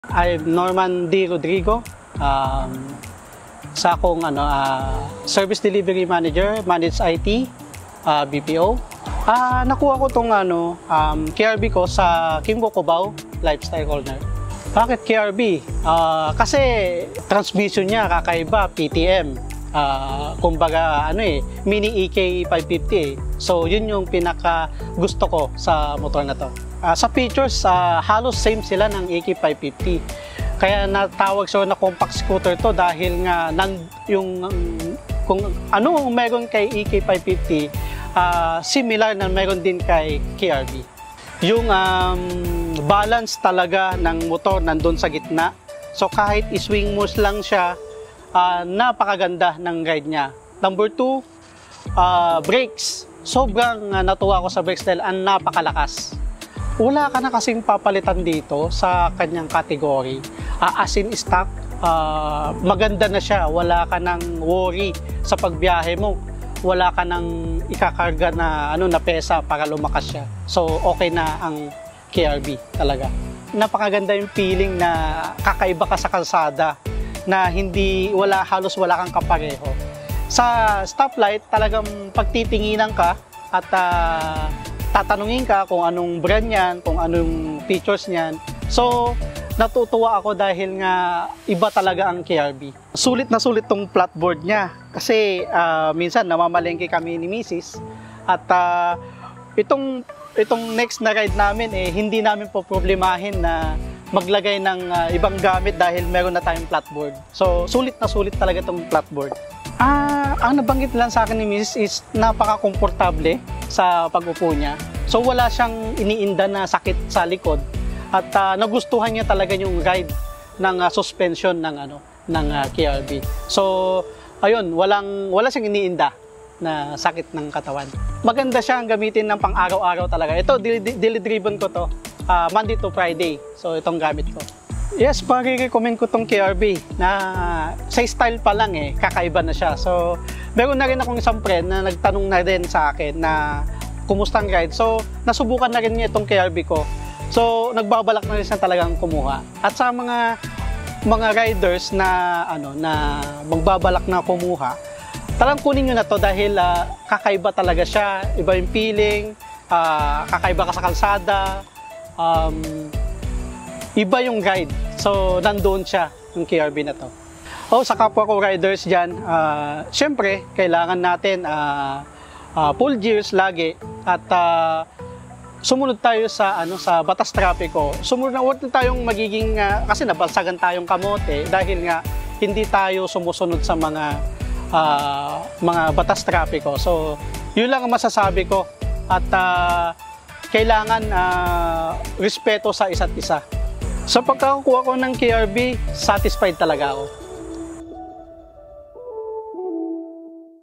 KYMCO KRV 180i | Testimonial Review of Mr. Normandi Rodrigo
Mr. Normandi Rodrigo chose the KYMCO KRV 180i as his new scooter. Why? Because he truly believes that the KYMCO KRV 180i's brand and features were derived from the KYMCO AK550.

"The KYMCO KRV 180i's performance was superb, and there was no need to upgrade any of it. When I stopped at a stop light and everyone asked me about my scooter, this unit truly caught their attention" Normandi said.

KYMCO KRV 180i SRP Php 189,900.